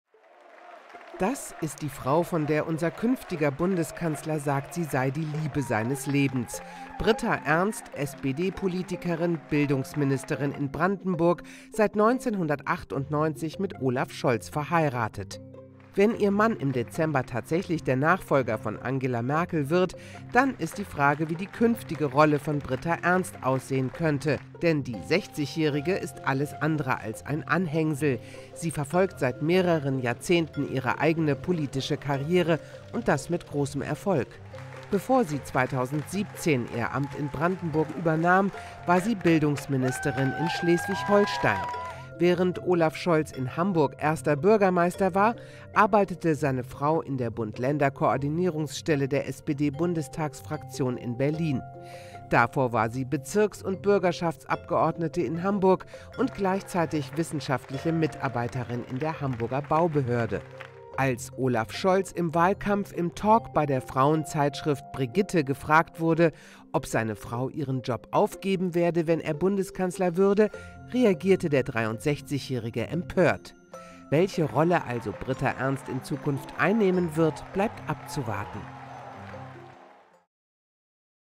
Britta Ernst: Erfolgreiche Politikerin statt klassischer First Lady | AFP
Die Liebe des Lebens von Olaf Scholz (SPD) und eine erfolgreiche Politikerin: Britta Ernst ist seit 1998 mit dem voraussichtlich nächsten Bundeskanzler verheiratet. Derzeit ist sie Bildungsministerin in Brandenburg.